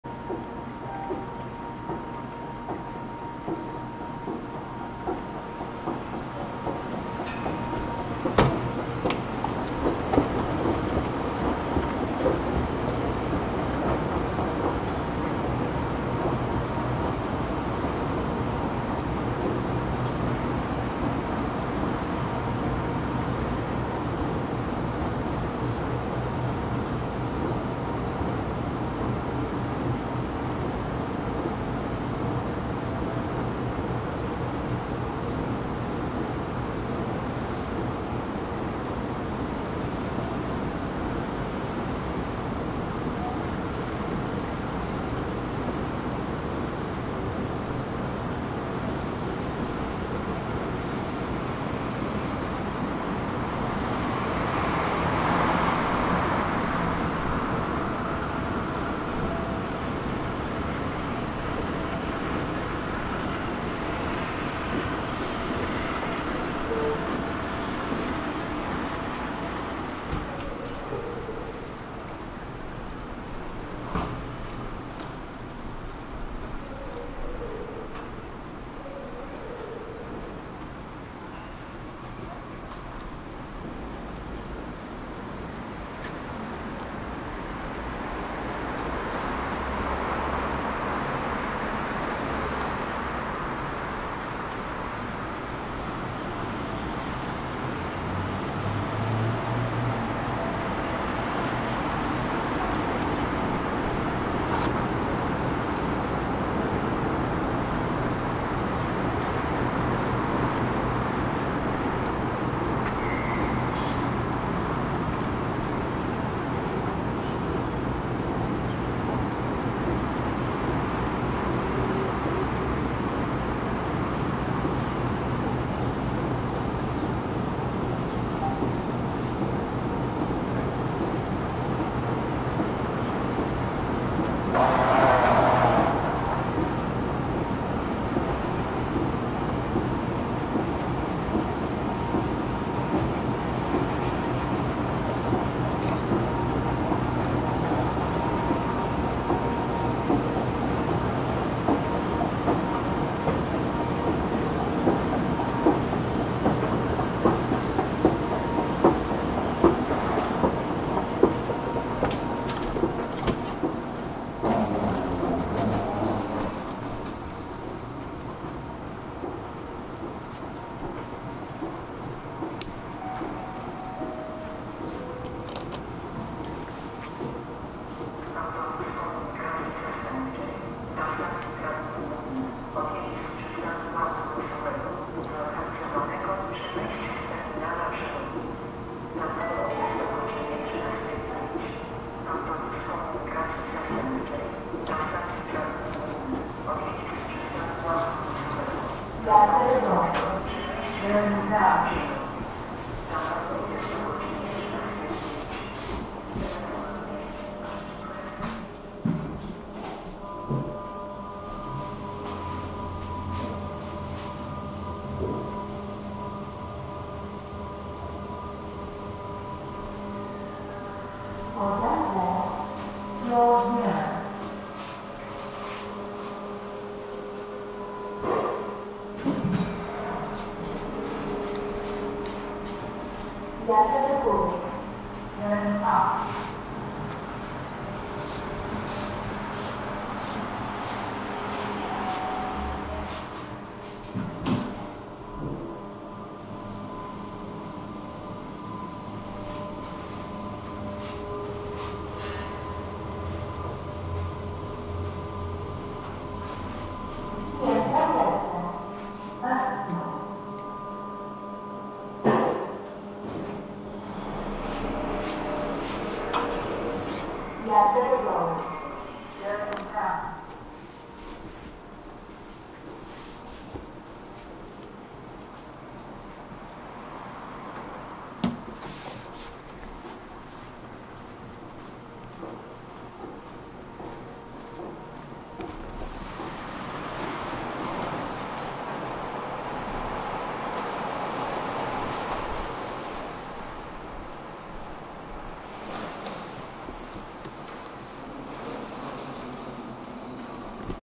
Poland, Kraków Airport railway station, 2X escalator, 1X elevator
escalator 0:04 , 1:38

elevator 2:46

- recorded with Sony a7III (firmware 3.01), Sony FE 24mm F1.4 GM, Pilotfly Adventurer, Sony ECM-XYST1M

July 2019